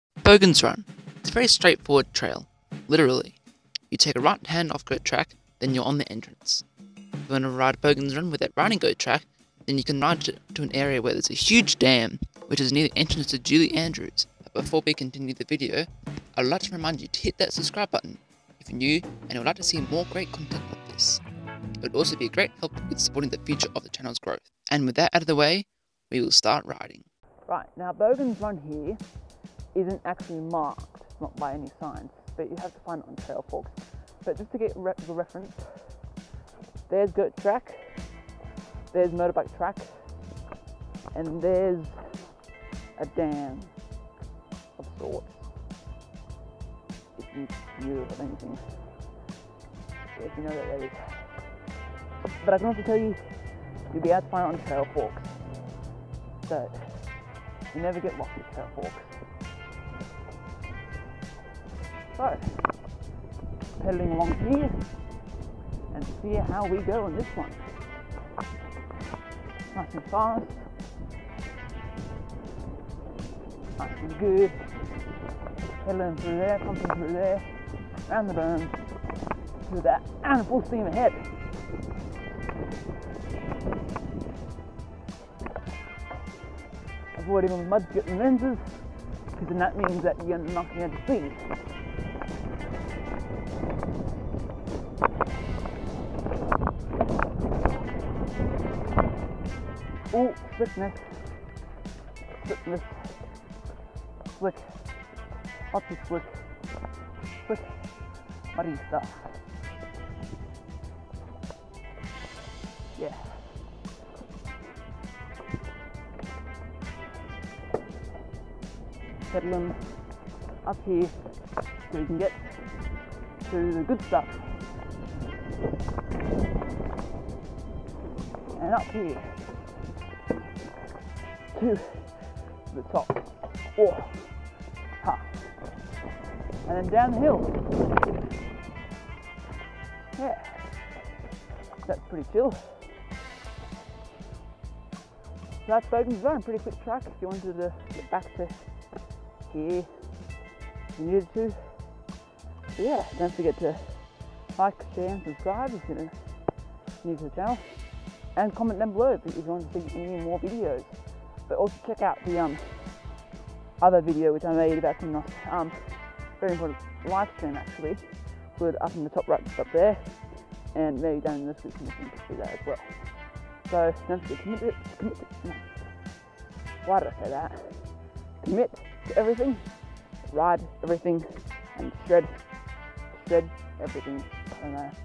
Kinross MTB Park (Bogans Run) "Blue Square"
- Kinross MTB Park has a great track called, Bogans run it's a very straight forward trail...literally. It's not marked with any signs, although it's in an area where there's a huge dam, it's also on the exit of motorbike track and goat track.
Once you reach the end of a few hundred meters of flow then there's a great rock slab to finish off the track.

- Things I use to film and edit:
     - My Cameras:
Kasier Baas x150